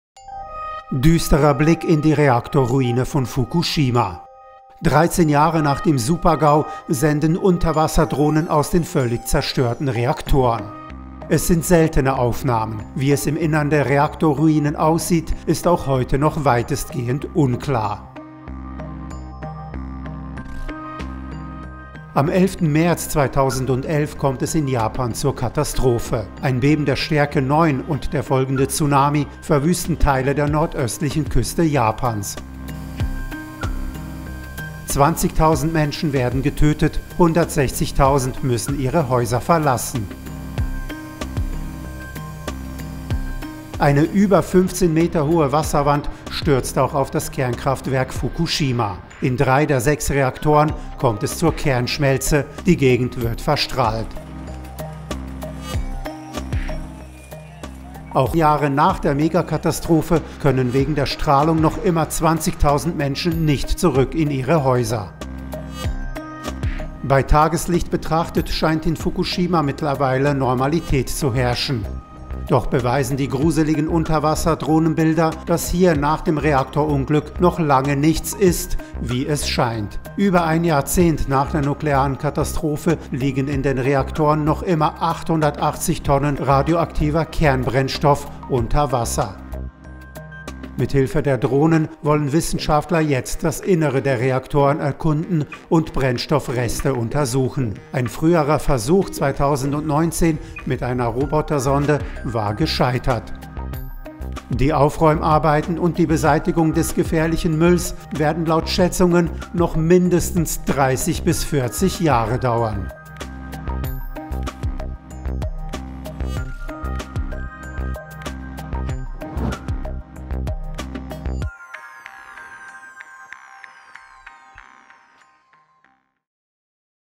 Drohne filmt in Fukushimas Reaktor-Ruine
Düsterer Blick in die Reaktor-Ruine von Fukushima. 13 Jahre nach dem Super-GAU sendet eine Unterwasser-Drohne aus dem völlig zerstörten Reaktor.